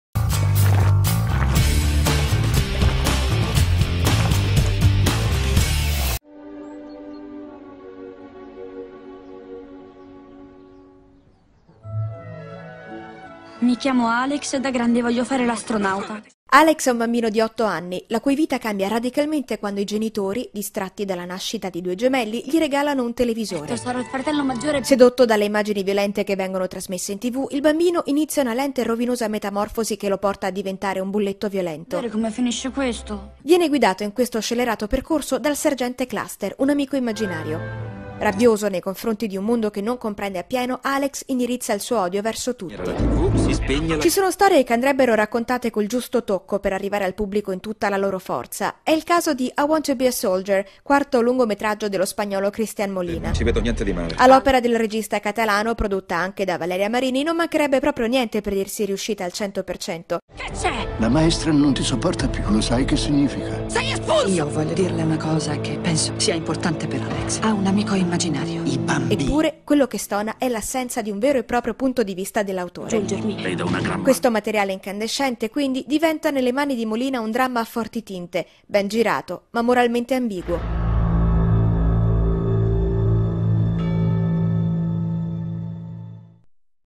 I Want to Be a Soldier - Video-recensioni di Movieplayer.it
La videorecensione del film I Want to Be a Soldier di Christian Molina, con Fergus Riordan, Ben Temple e Danny Glover. Testi di Francesca Fiorentino e voce di Francesca Fiorentino.